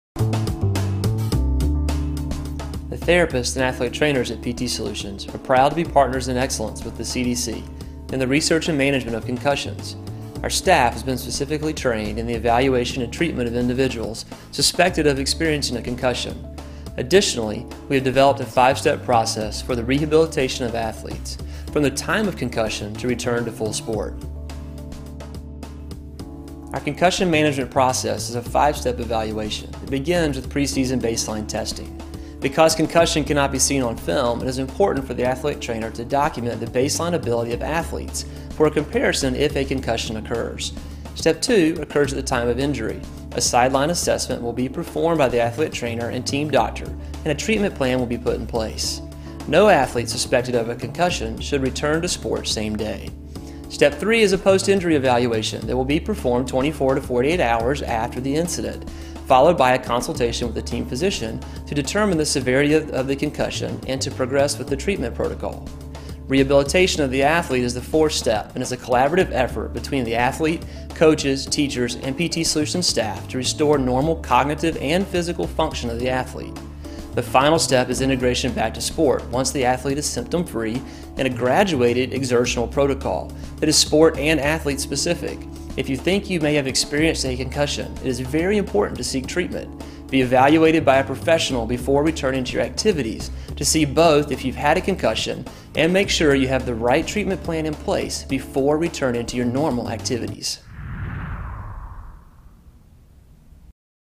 PT Solutions - Five Step Concussion Evaluation Process
Daniel Pernell, PT MBA, walks us through the five step concussion evaluation process developed by PT Solutions.